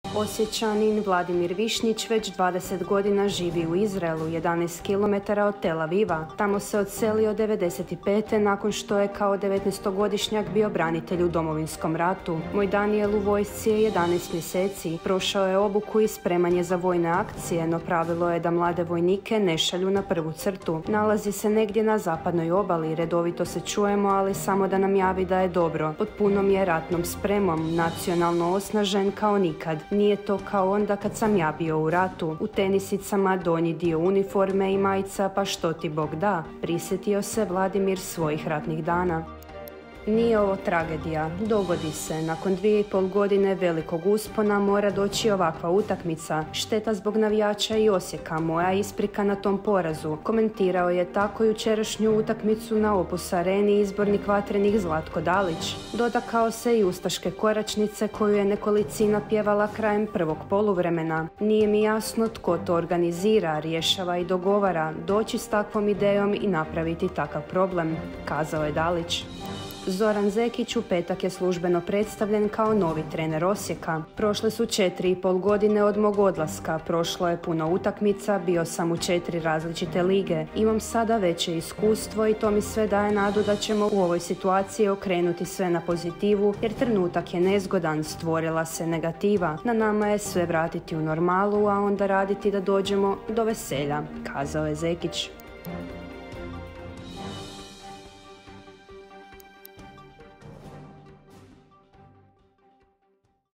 Osječanin u Tel Avivu za 24sata: 'Moj sin brani Izrael baš kao što sam ja nekada branio Hrvatsku'
Osječanin u Tel Avivu za 24sata: 'Moj sin brani Izrael baš kao što sam ja nekada branio Hrvatsku' I TOP 3 VIJESTI DANA

Imate priču?